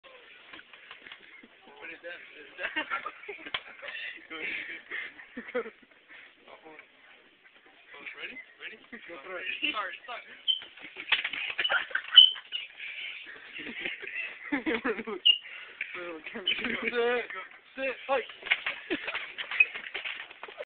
Max, Chuck & Poncho Getting Down At Walmart
These Fukn Randoms Playing Football